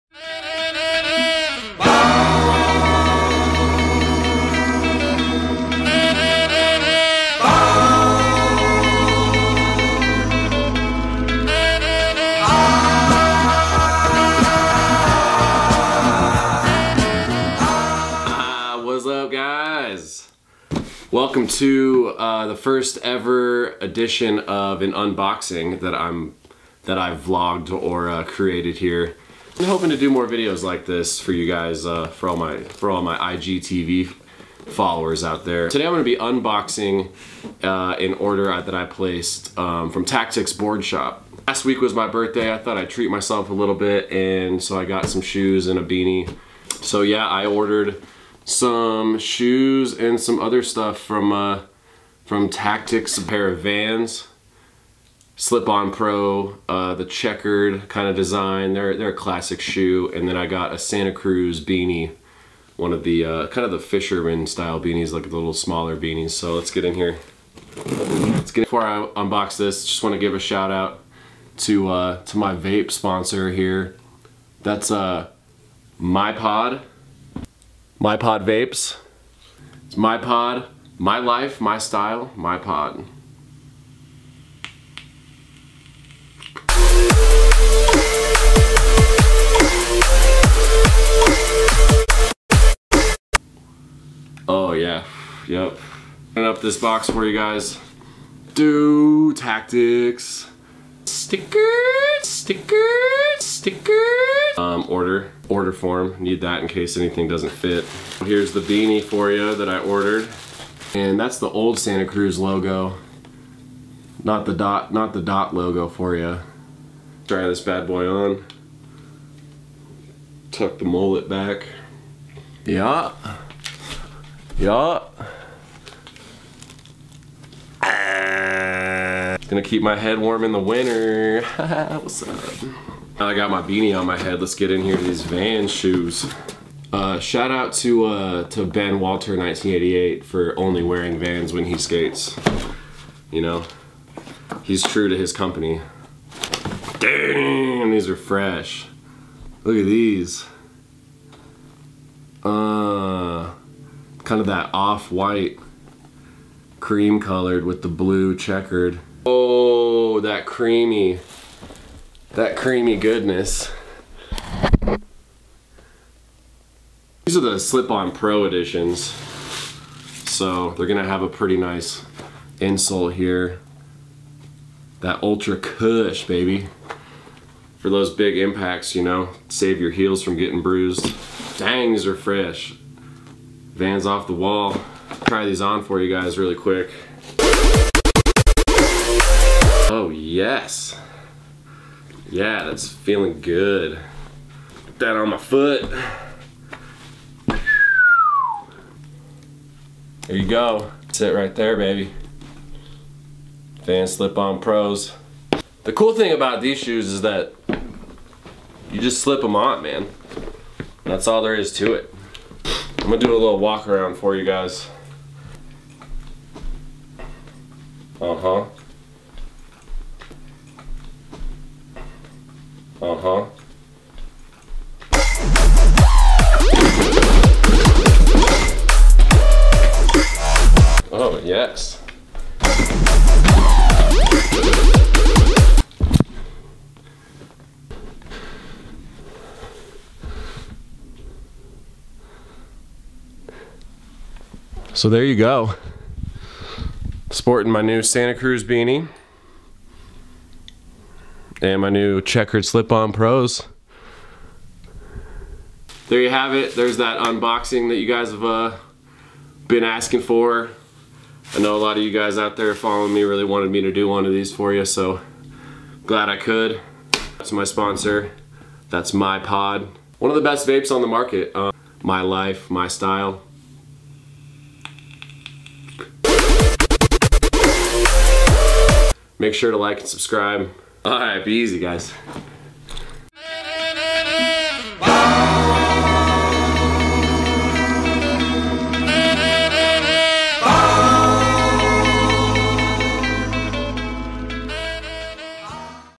Unboxing Vans Slip-ons
Just goofin' off with my new order of Vans shoes and Santa Cruz beanie